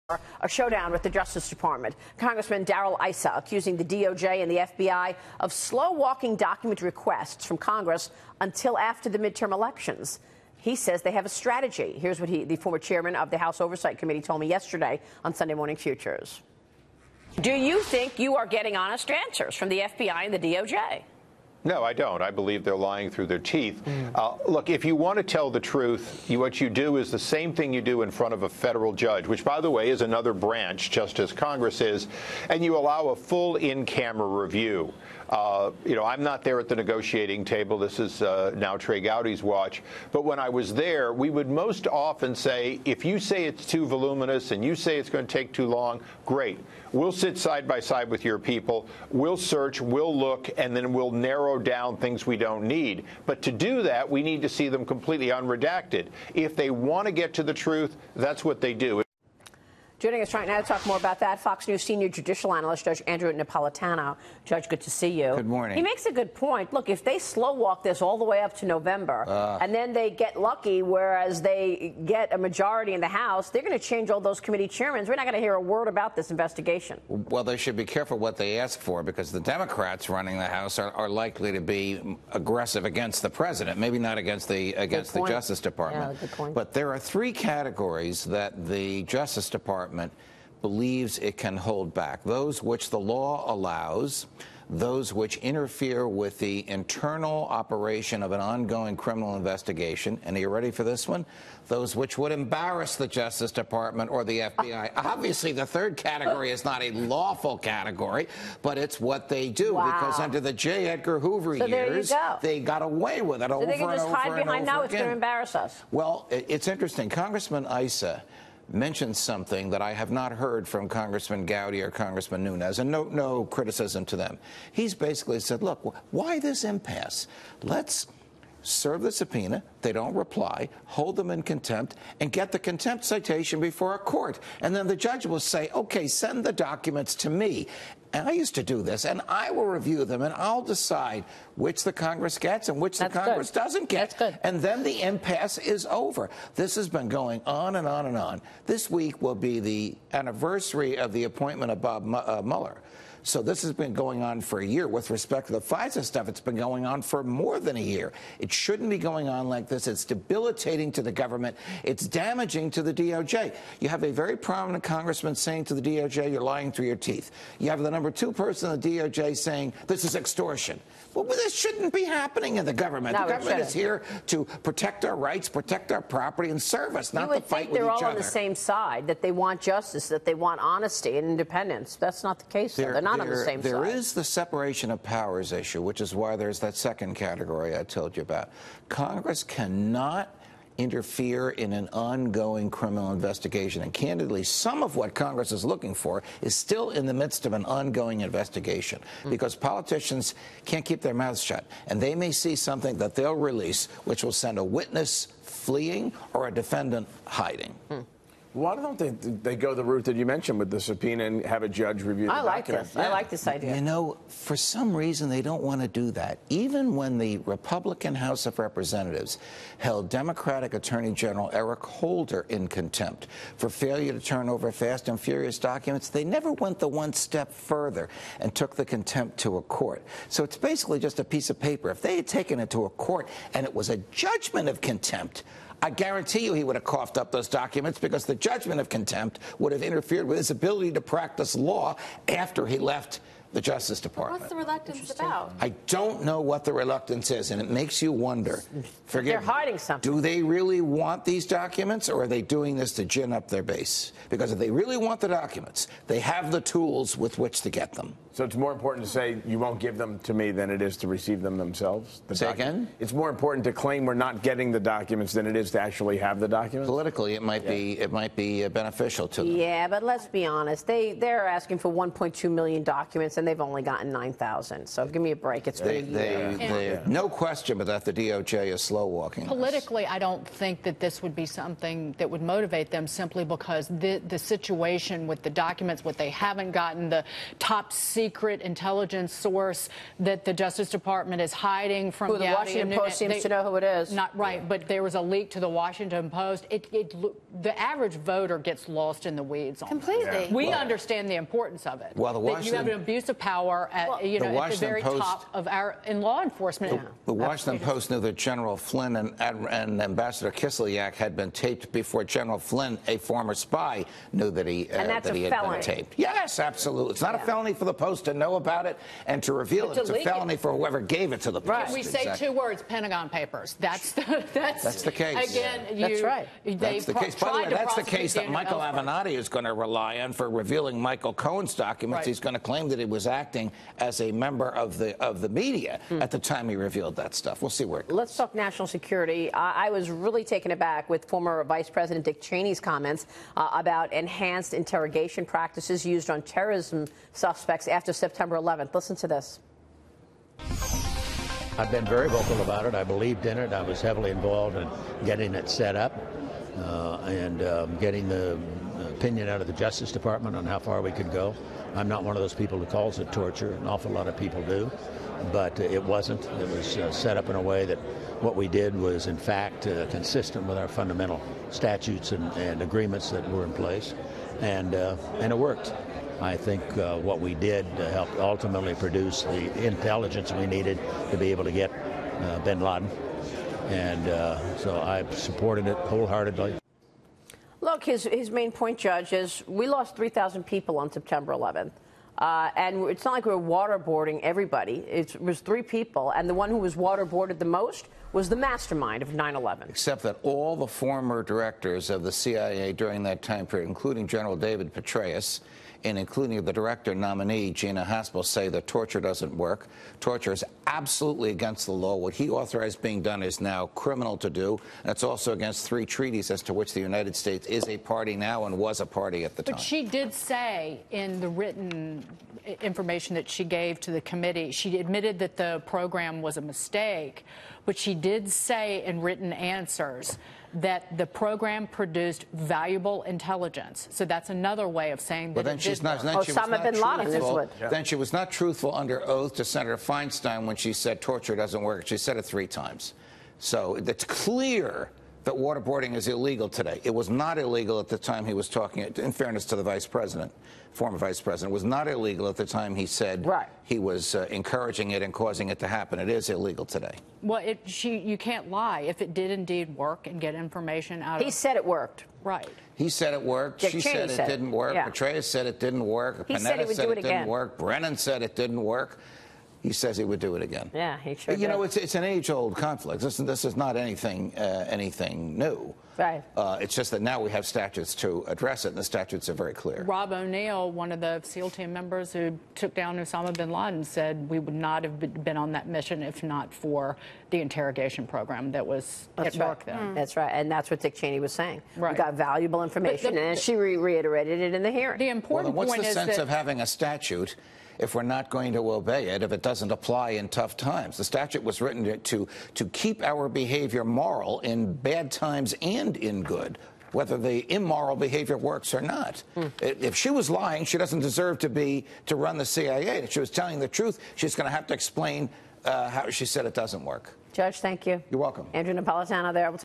No question that the DOJ is slow walking: Judge Napolitano
Fox News Senior Judicial Analyst Judge Andrew Napolitano on the DOJ and FBI's response to lawmakers' requests for documents and CIA Director nominee Gina Haspel's comments on enhanced interrogation.